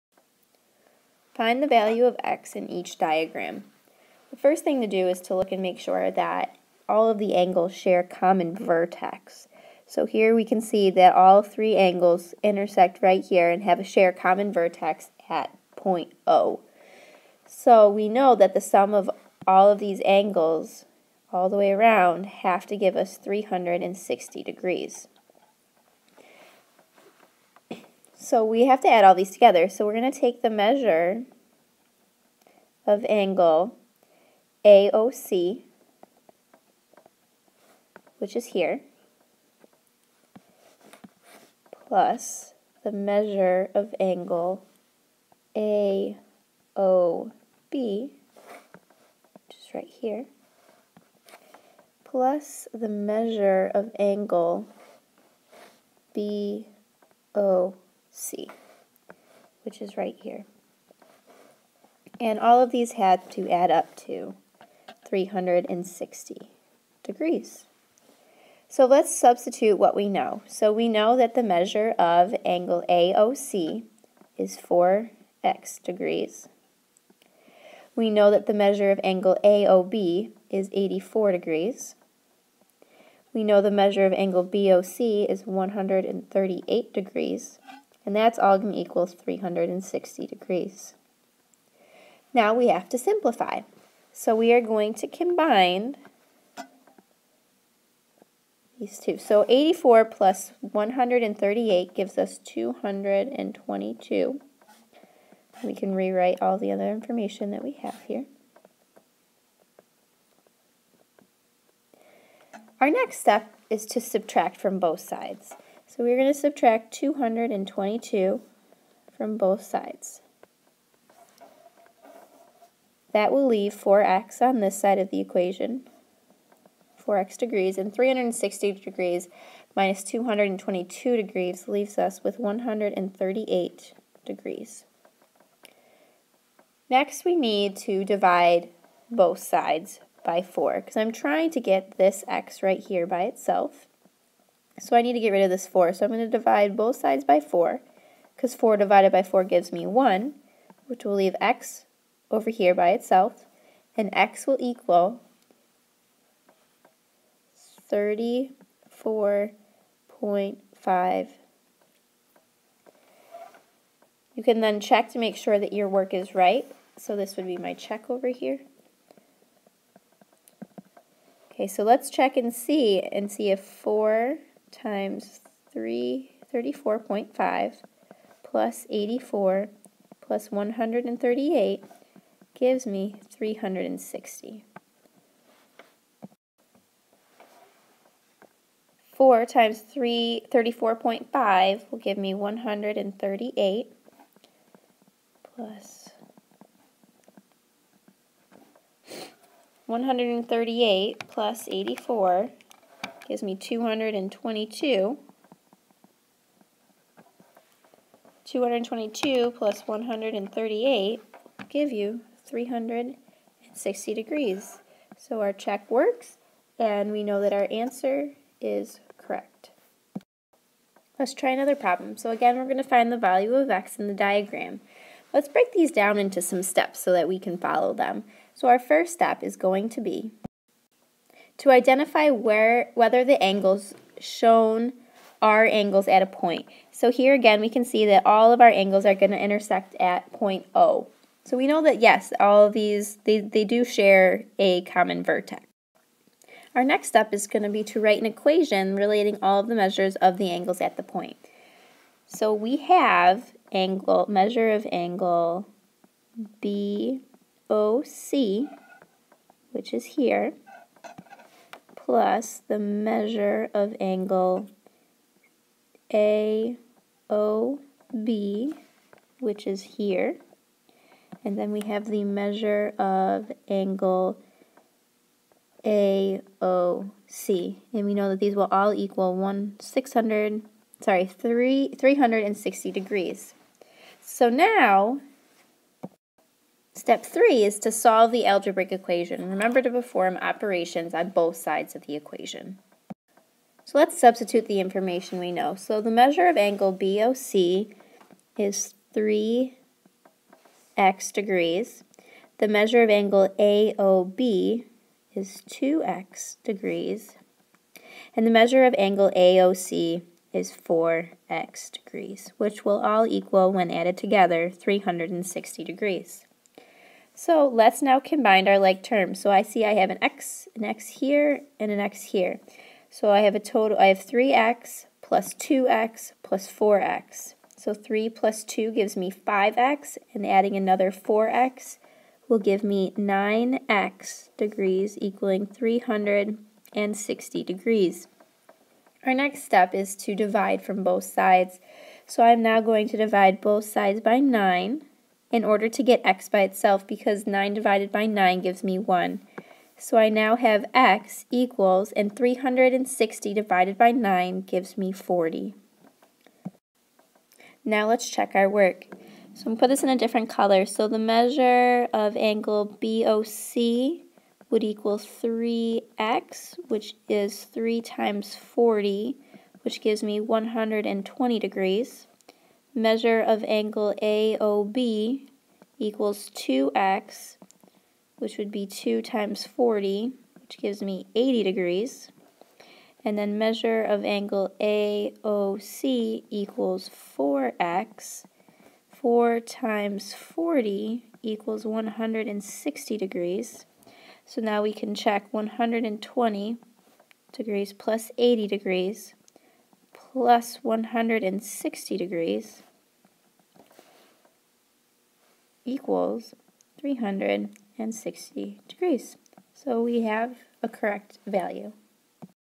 Use algebra to solve problems involving angles at a point.
Worked example from Math In Focus Course 2B page 20-21.